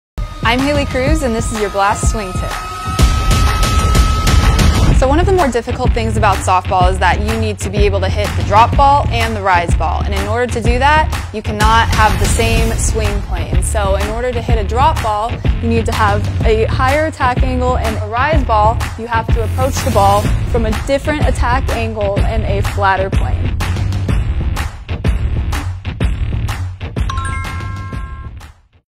Haley Cruse Swing Tip - Swing Plane & Attack Angle
Professional softball player for the USSSA Pride and Blast Motion ambassador Haley Cruse explains how pitch height impacts your attack angle and swing plane!